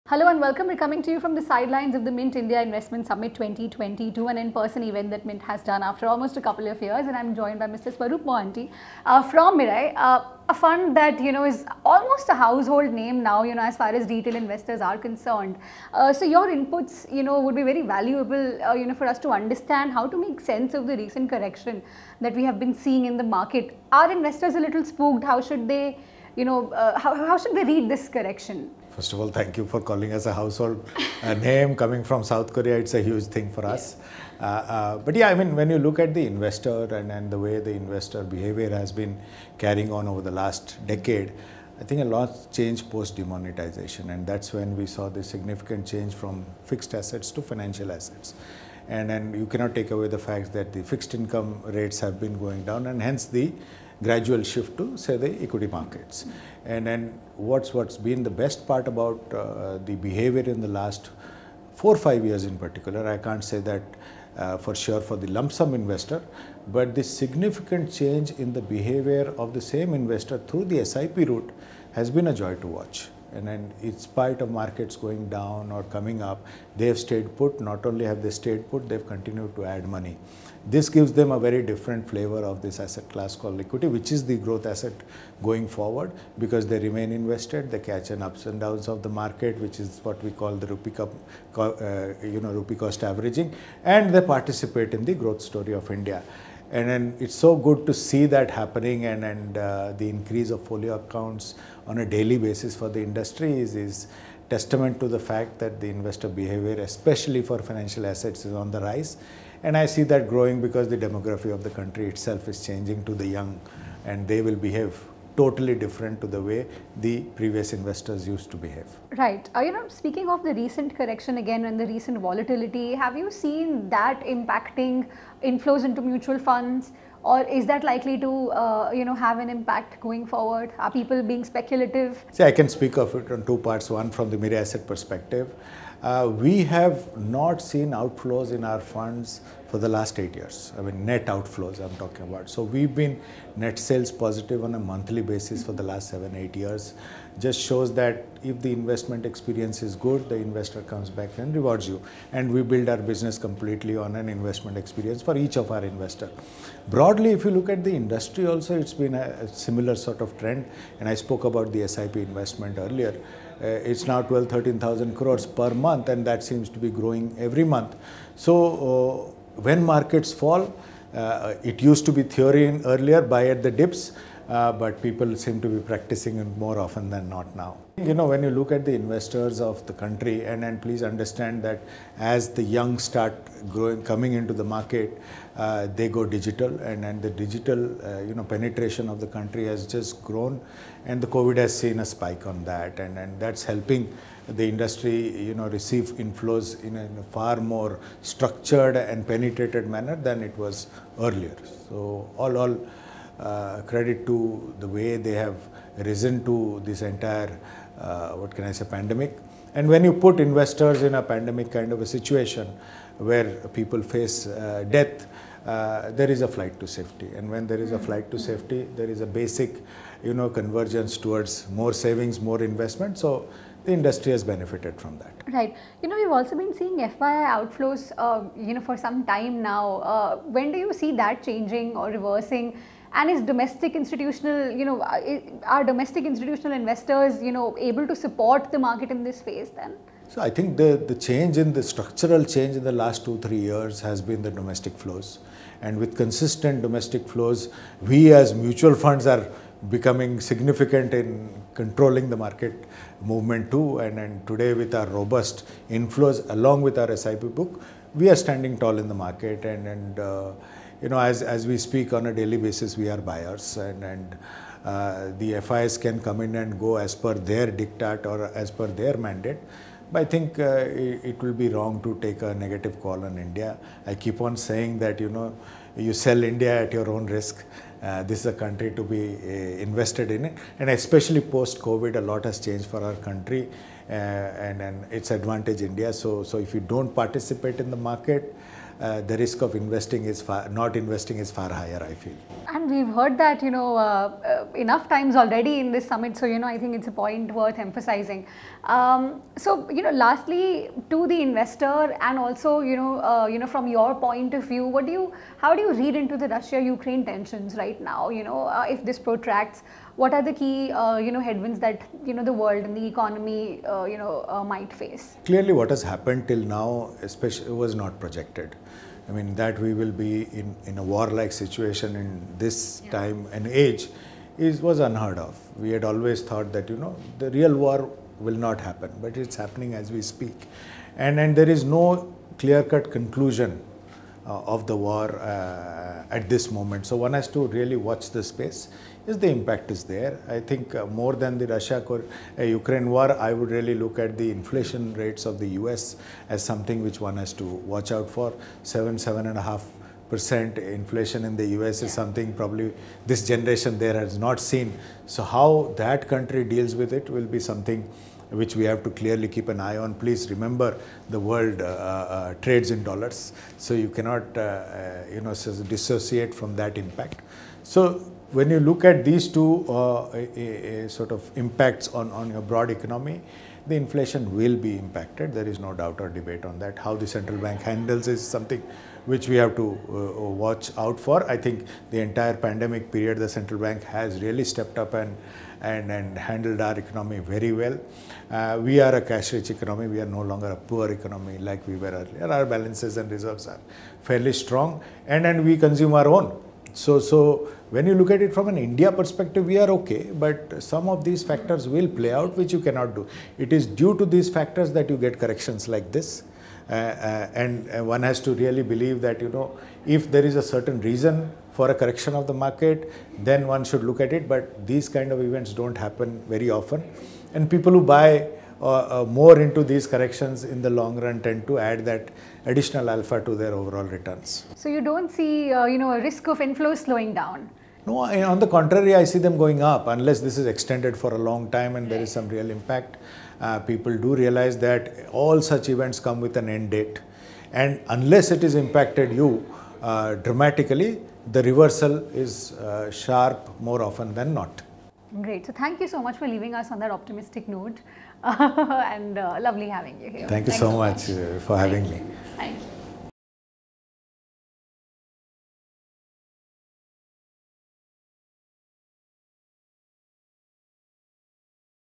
‘People seem to be practicing ‘buy the dip’ more than ever’: Mirae Asset’s Swarup Mohanty
Mirae Asset Investment Managers CEO Swarup Mohanty tells Alisha Sachdev, Assistant Editor, Mint, that the retail investor is ‘buying the dip’ more than ever – in the backdrop of rapid digitsation and a pandemic-induced flight to safety, the mutual fund industry has benefited from an increase in inflows in a far more structured and penetrated manner. Mohanty adds that retail investors are choosing to stay put with their investments, and are even doubling down on investing in mutual funds. As a result, they’re able to reap the benefits of seeing the markets through its rises and falls and create wealth in the process. He added that Mirae Asset has not seen net outflows in their funds for the last 7-8 years.